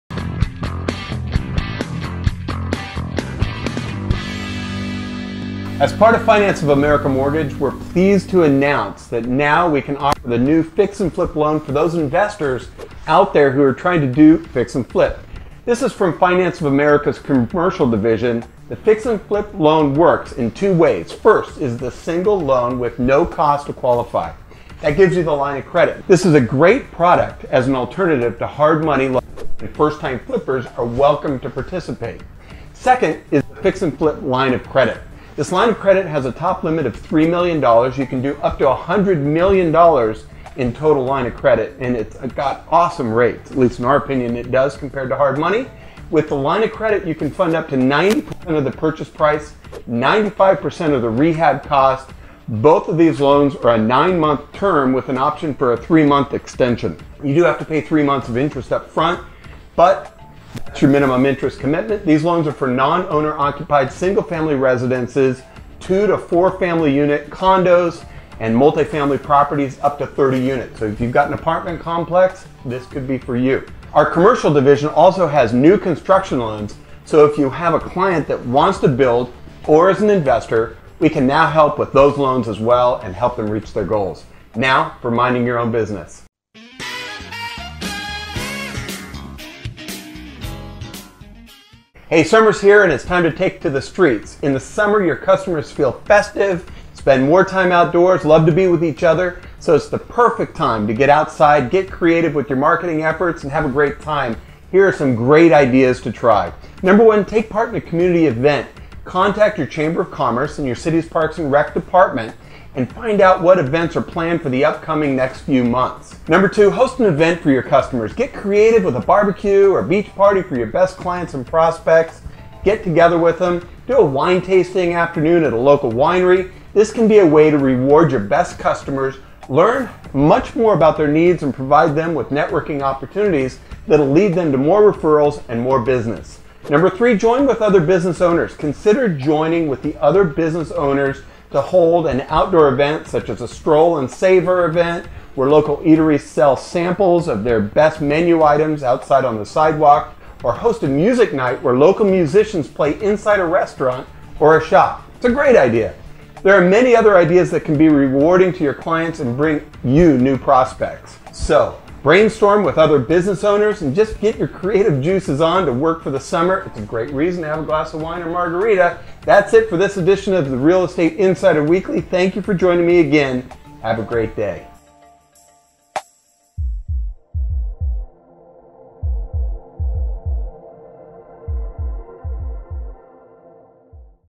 The Fix and Flip Loan Products for the Investors
As part of Finance of America Mortgage, we are pleased to announce that now can offer the new Fix and Flip Loan for those investors out there, from Finance of America Commercial!  The Fix and Flip Loan works in two ways. First is the single loan, and second is the line of credit... check it out!

This week's Marketing Tip is about using creativity for great summer marketing events!

Kevin Fritz
MLO #220254
Iron Point Mortgage